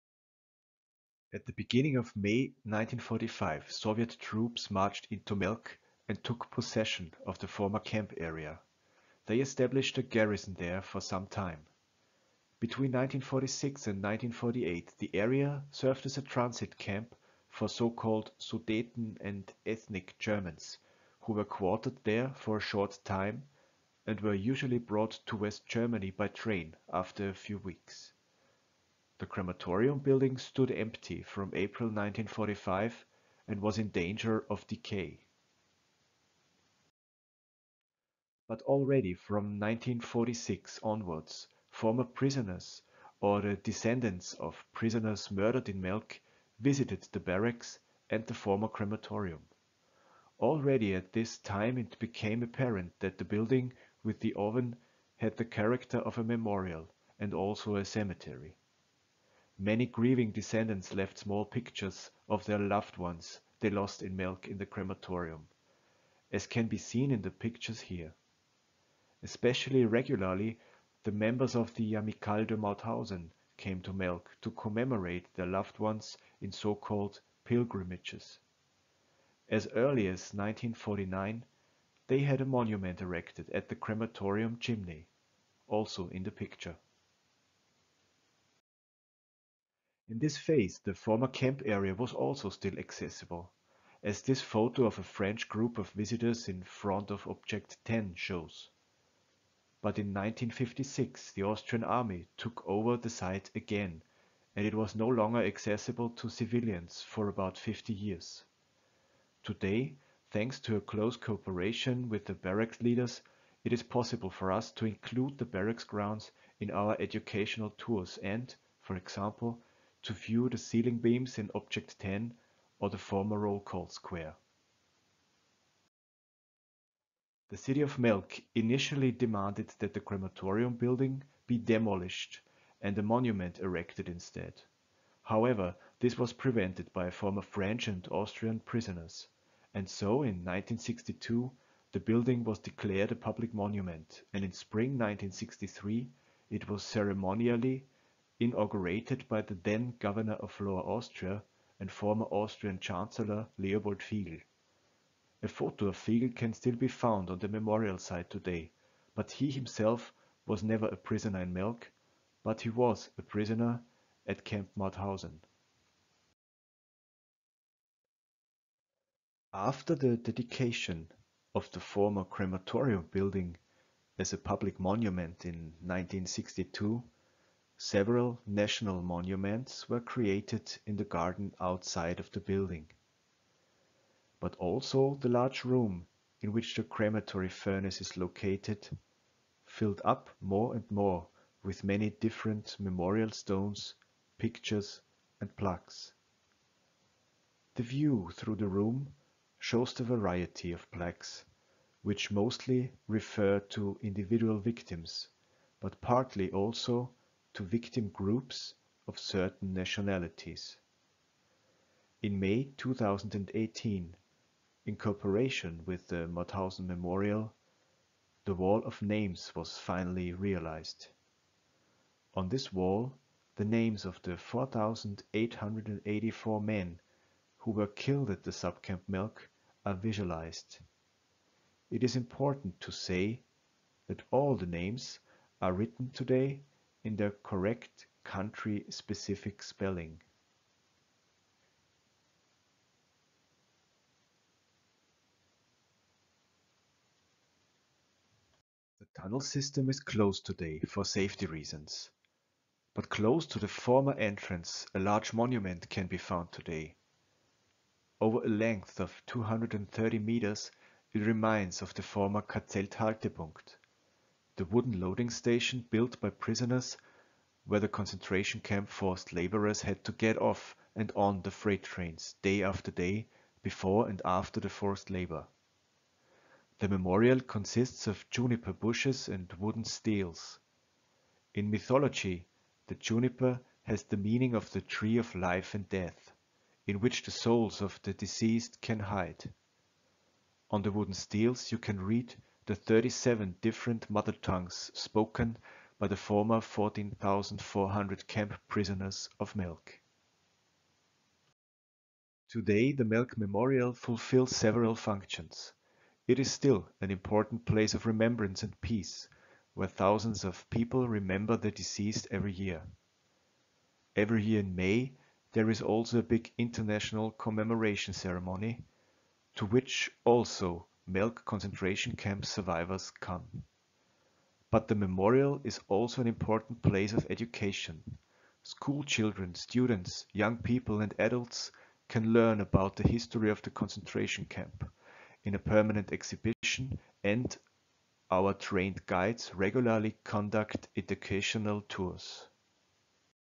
Virtual Tour Subcamp Melk - Station 10: History of the Melk Memorial
At the beginning of May 1945 Soviet troops marched into Melk and took possession of the former camp area. They established a garrison there for some time. Between 1946 and 1948 the area served as a transit camp for so called Sudeten and ethnic Germans who were quartered there for a short time and were usually brought to West Germany by train after a few weeks. The crematorium building stood empty from April 1945 and was in danger of decay.
But already from 1946 onwards former prisoners or the descendants of prisoners murdered in Melk visited the barracks and the former crematorium. Already at this time it became apparent that the building with the oven had the character of a memorial and also a cemetery. Many grieving descendants left small pictures of their loved ones they lost in Melk in the crematorium, as can be seen in the pictures here. Especially regularly the members of the Amicale de Mauthausen came to Melk to commemorate their loved ones in so-called pilgrimages. As early as 1949, they had a monument erected at the crematorium chimney - also in the picture.
In this phase, the former camp area was also still accessible, as this photo of a French group of visitors in front of object 10 shows. But in 1956 the Austrian army took over the site again and it was no longer accessible to civilians for about 50 years. Today, thanks to close cooperation with the barracks leaders, it is possible for us to include the barracks grounds in our educational tours and, for example, to view the ceiling beams in Object 10 or the former roll call square.
The city of Melk initially demanded that the crematorium building be demolished and a monument erected instead. However, this was prevented by former French and Austrian prisoners. And so, in 1962, the building was declared a public monument and in spring 1963, it was ceremonially inaugurated by the then Governor of Lower Austria and former Austrian Chancellor Leopold Figl. A photo of Figl can still be found on the memorial site today, but he himself was never a prisoner in Melk, but he was a prisoner in Mauthausen.
After the dedication of the former crematorium-building as a public monument in 1962, several national monuments were created in the garden outside of the building. But also the large room, in which the crematory furnace is located, filled up more and more with many different memorial stones, pictures and plaques. The view through the room shows the variety of plaques, which mostly refer to individual victims, but partly also to victim-groups of certain nationalities. In May 2018, in cooperation with the Mauthausen Memorial, the Wall of Names was finally realized. On this wall the names of the 4,884 men, who were killed at the subcamp Melk are visualized. It is important to say, that all the names are written in their correct country-specific spelling.
The tunnel system is closed today for safety reasons. But close to the former entrance a large monument can be found today. Over a length of 230 metres it reminds of the former "KZ-Haltepunkt" - the wooden loading station built by prisoners where the concentration camp forced labourers had to get off and on the freight trains day after day before and after the forced labour. The memorial consists of juniper bushes and wooden steles. In mythology, the juniper has the meaning of the tree of life/death, in which the souls of the deceased can hide. On the wooden steles you can read the 37 different mother tongues spoken by the former 14.400 camp-prisoners of Melk.
Today the Memorial Melk fulfils several functions. It is still an important place of remembrance and peace, where thousands of people remember the deceased every year. Every year in May there is also a big international commemoration ceremony, to which also every year Melk concentration camp survivors come. But the memorial is also an important place of education. Schoolchildren, students, young people and adults can learn about the history of the concentration camp in a permanent exhibition and our trained guides regularly conduct educational tours.

Sources:
Picture 1: Melk Memorial, Center for contemporary history Melk, C. Rabl / Picture 2 – Composition: Center for contemporary history Melk, C. Rabl / Picture 3: Object 10, Robert Pachon, 1948 / Picture 4: Commemoration Melk Memorial 1963, NÖLA, Pressestelle, NLK Filzwieser, 1963 / Video: Center for contemporary history Melk, C. Rabl / Picture 6 – Composition: Center for contemporary history Melk, C. Rabl / Picture 7: Rene Baumann and Andrew Sternberg, Center for contemporary history Melk, C. Rabl.

Text: Center for contemporary history, C. Rabl